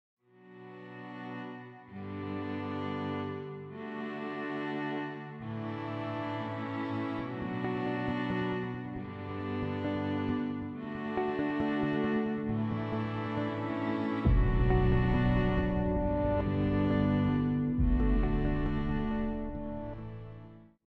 How to scrub through audio with a mouse in Hindenburg Pro
Manually control the scrub with a Mouse in your Hindenburg session.

Apple Shortcut: Cmd Mouse
Windows Shortcut: Ctrl mouse

Hindenburg PRO: Scrub - Mouse Scrub